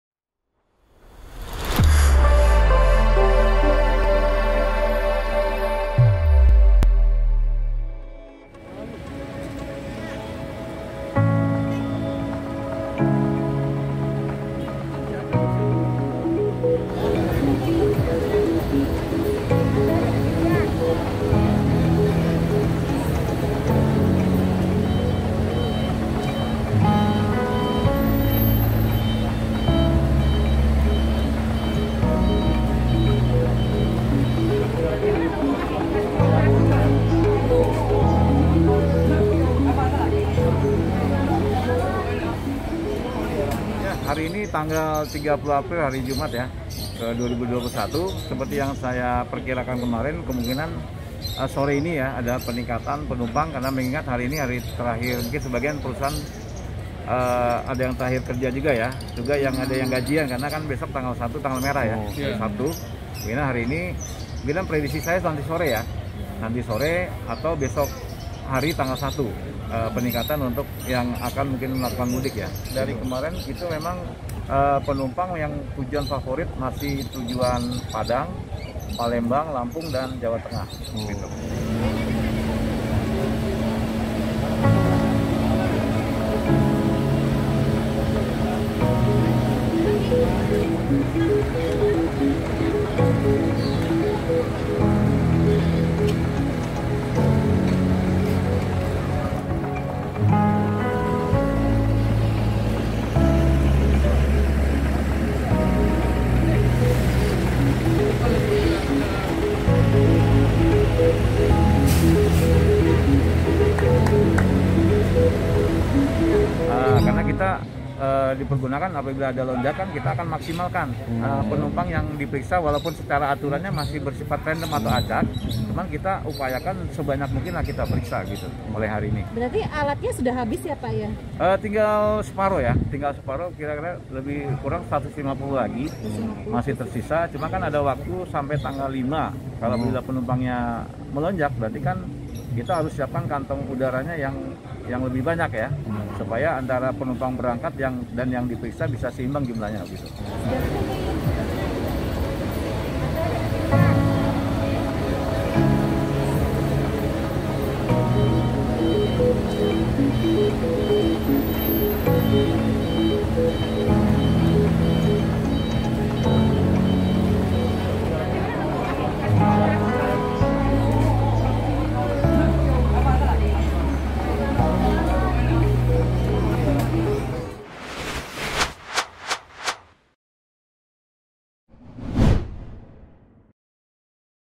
HARI TERAKHIR OPERASIONAL, PENUMPANG BUS DI TERMINAL KALIDERES MENINGKAT
JAKARTA, POSKOTA.CO.ID - Penumpang bus di Terminal Kalideres mengalami peningkatan pada dua minggu menjelang Lebaran Idul Fitri 1442 Hijriah. alami peningkatan meski belum terlalu siginifikan.

"Kemungkinan sore ini ada peningkatan penumpang mengingat hari ini hari terakhir," kata Kepala Terminal Kaliderez Revi Zulkarnaen kepada poskota, Jumat (30/4/2021).

Revi menuturkan peningkatan jumlah penumpang bus di terminal Kalideres lantaran sebagain perusahaan sudah meliburkan karyawannya.
"Ada yang udah gajian juga dan besok tanggal 1 tanggal merah. Prediksi saya nanti sore atau besok tanggal 1 peningkatan mudik lebaran," jelasnya.

Di terminal kalideres sendiri, Revi mengatakan bahwa sudah membatasi jumlah penumpang bus sebanyan 50 persen.

Para penumpang bus juga diwajibkan untuk menaati protokol kesehatan seperti memakai masker dan menjaga jarak serta mencuci tangan.

Sementara terkait pemeriksaan GeNose kepada calon penumpang, masih dilakukan secara random atau acak.

"Kita targetkan satu hari 15 penumpang, kalau ada peningkatan kita akan maksimalkan sampai berapa kali lipat supaya penumpang yang berangkat dan diperiksa seimbang," paparnya.

Jika calon penumpang hasilnya positif saat di tes GeNose, maka akan dilakukan tes PCR oleh pihak Puskesmas Kalideres.

"Petugas medis Puskesmas Kalideres sudah standby disini," tandasnya.